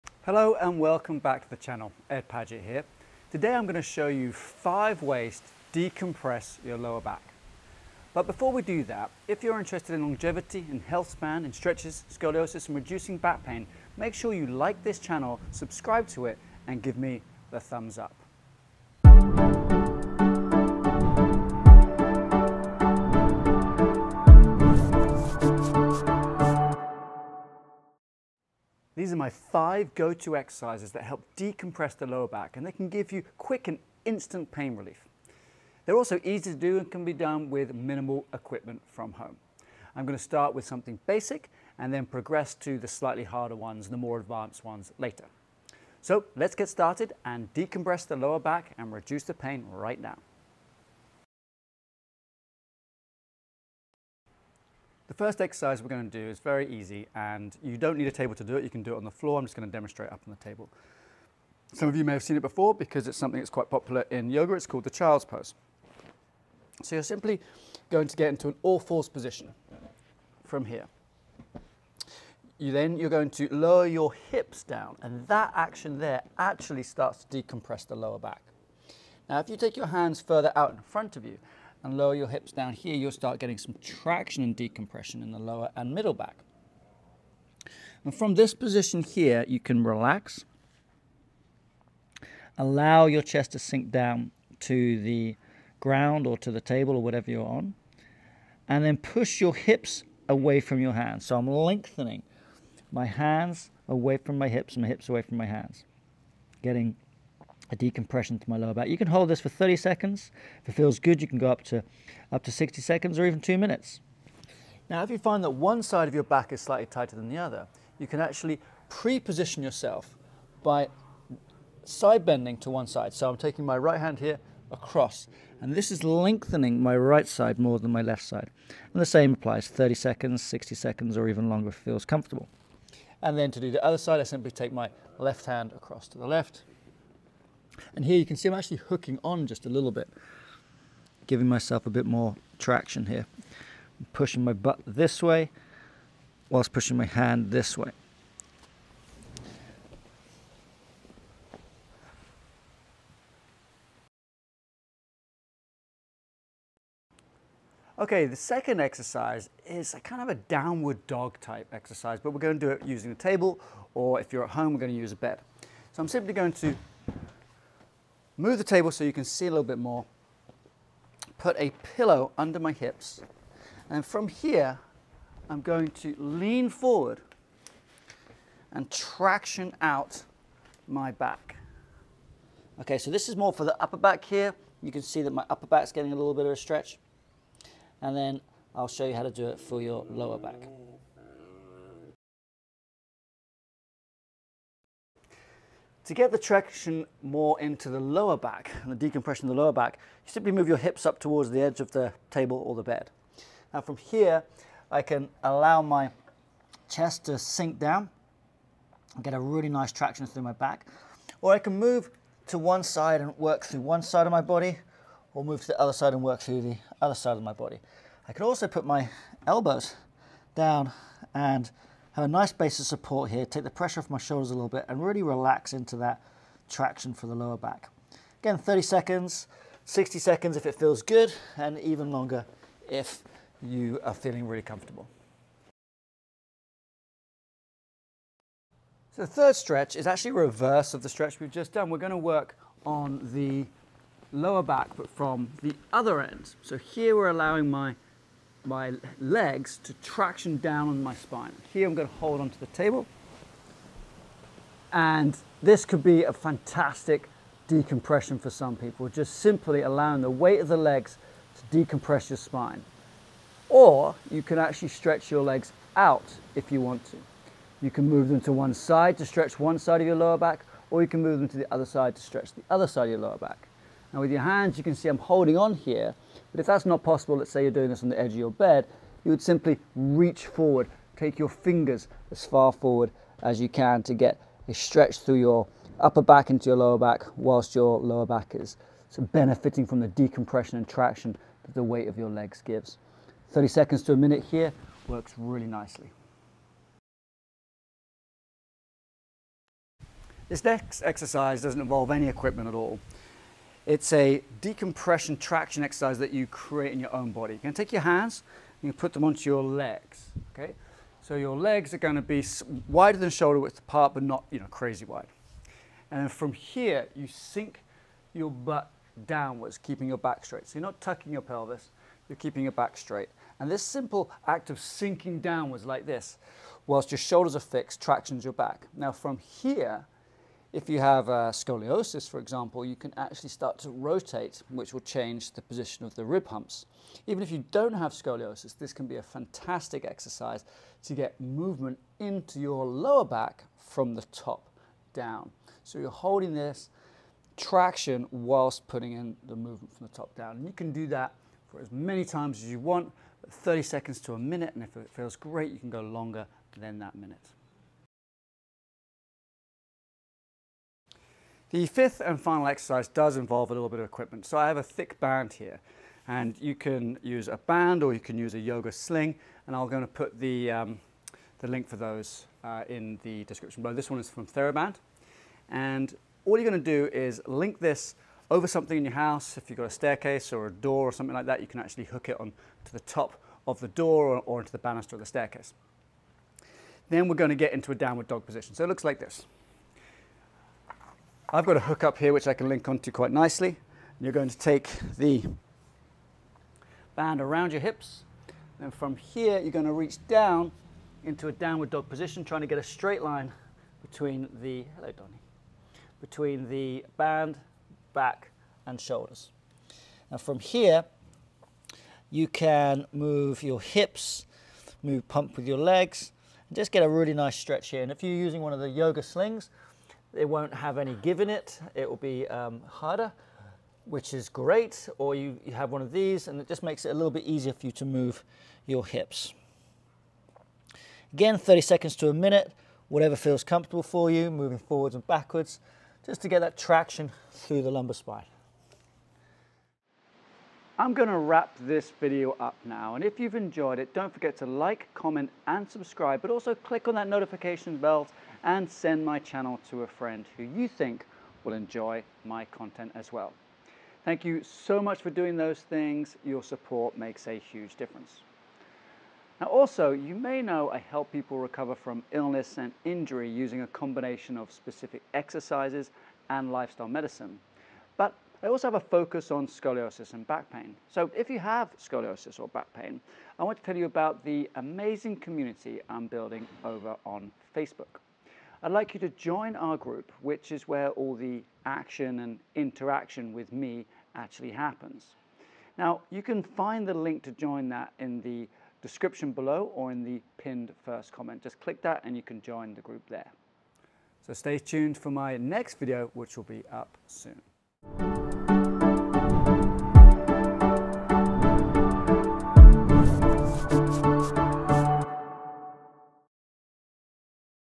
5 Ways To Decompress Your Lower Back | Instant Relief at HOME
These are my 5 go-to exercises that help decompress the lower back and can give you quick and instant pain relief.

They're also easy to do and can be done with minimal equipment from home. I will start with something basic and then progress to the slightly harder ones and the more advanced ones later.

Exercises
1. Child-pose with different reaches, do 30 seconds; if you are comfortable, go for 60.
2. Edge of bed Downward dog decompression, play with arm position
3. Reverse it so the chest is on the table. Reach with arms over the bed like in the thumbnail.
4 Decompression standing.
5. Decompression with a band on the wall.

To talk to Ed or one of his team members, go to the following:

TIMETAGS
00:00 Intro
00:51 Child’s Pose (30 s to 1 min)
02:37 Modified downward dog
03:12 Lower back emphasis
03:57 Lower back traction
04:57 Standing traction
05:58 Downward dog with traction
07:21  Outro